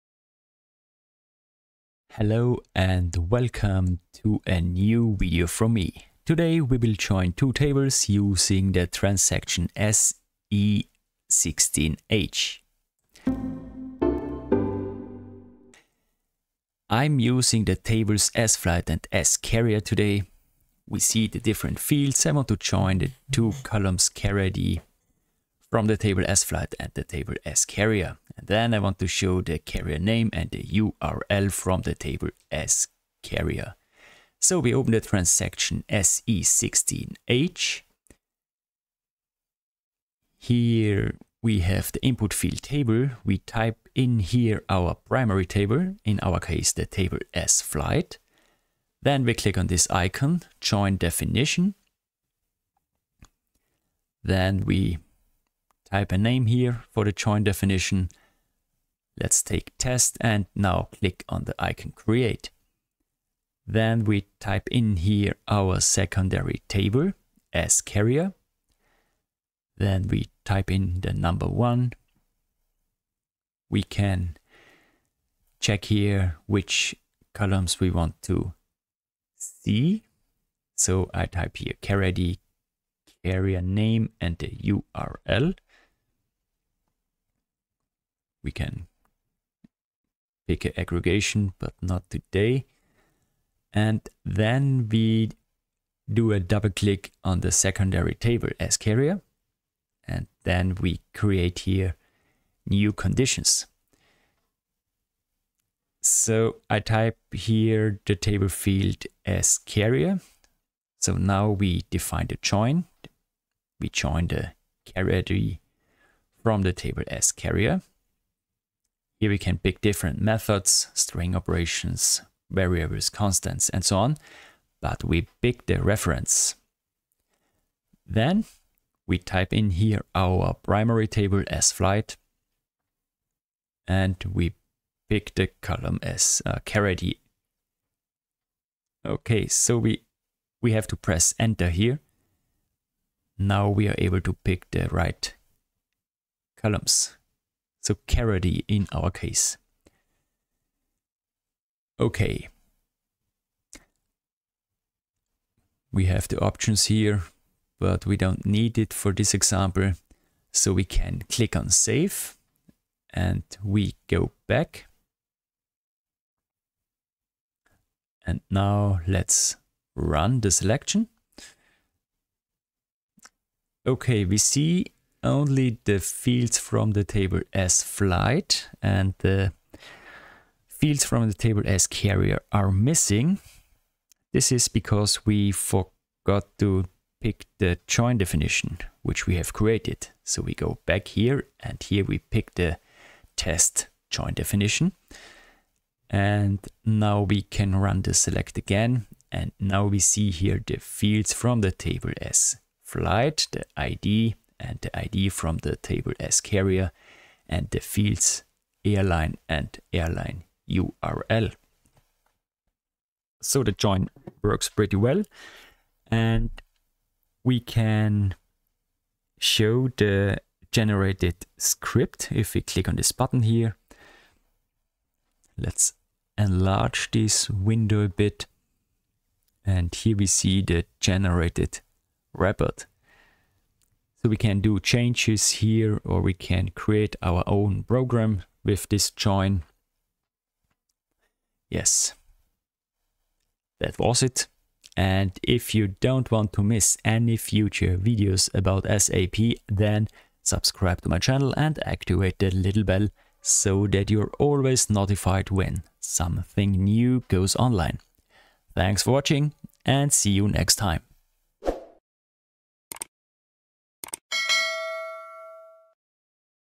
Mastering SAP SE16H: Table Joins Made Easy [english]
Today, we will join two tables using transaction SE16H.

▬▬  Literature  📚  ▬▬▬▬▬▬▬▬▬▬▬▬▬
▶ ABAP: An Introduction 2020
▶ Complete ABAP 2023
▶ ABAP to the Future 2022
▶ Clean ABAP 2022